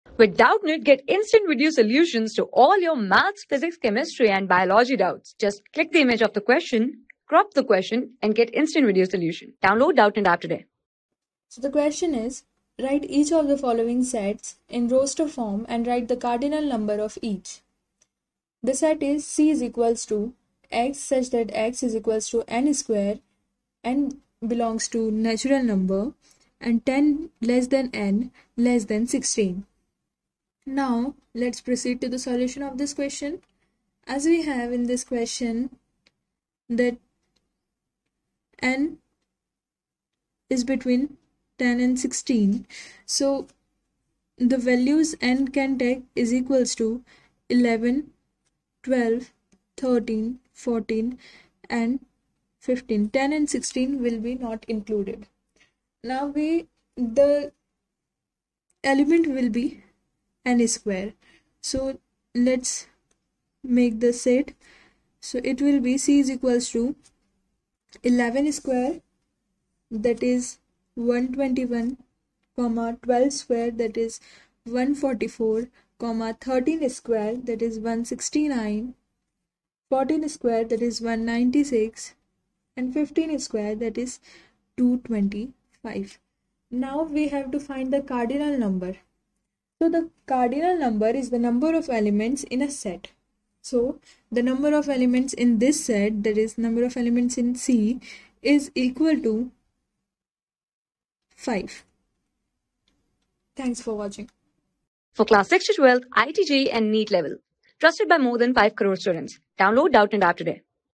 Write each of the following sets in Roster form and write the cardinal number of each .C = { x: ...
Write each of the following sets in Roster form and write the cardinal number of each .C = { x: x= n^(2) , n in N, 10 lt n lt 16}
Class: 7
Subject: MATHS
Chapter: SETS
Board:ICSE

👉 Have Any Query? Ask Us.